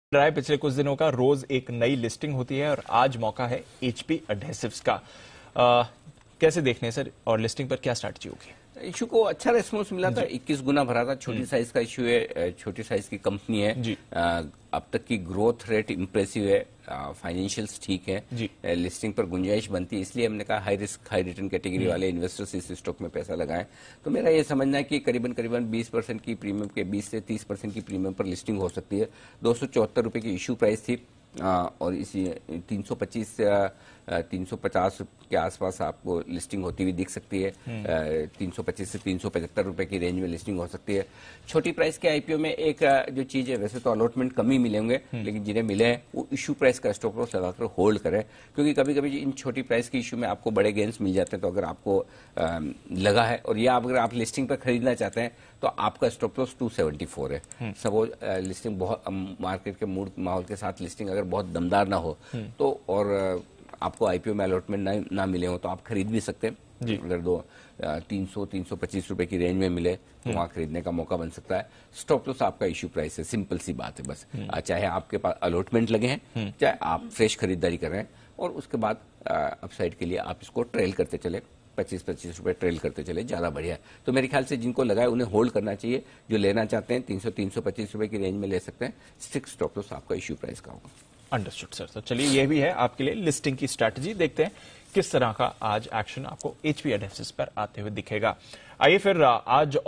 HP Adhesives listing today | Is a strong debut on the cards?
आज HPAdhesives की लिस्टिंग, इश्यू प्राइस ₹274

किस प्राइस पर हो सकती है लिस्टिंग?

लिस्टिंग के बाद क्या करें इन्वेस्टर्स?

कहां पर रखें स्टॉपलॉस?

जानिए अनिल सिंघवी की राय...